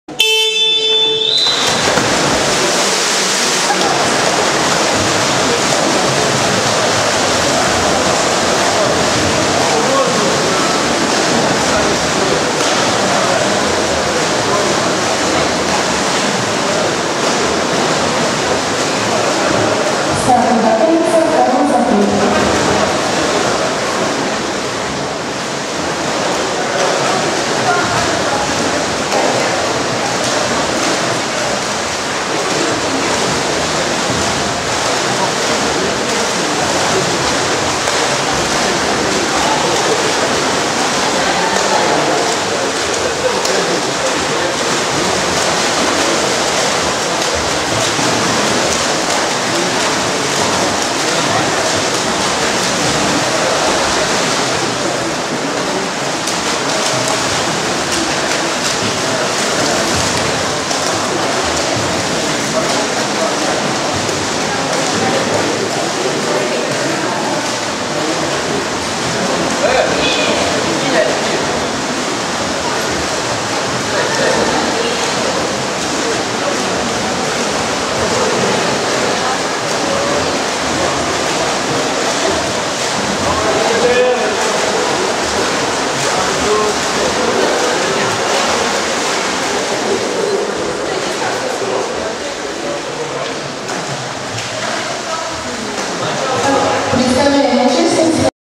Полиатлон_Тамбов-2011, плавание, женщины, заплыв № 1
Полиатлон, Кубок мира по четырехборью, Тамбов
29 января 2011 г., плавание 100 м, женщины
Заплыв № 1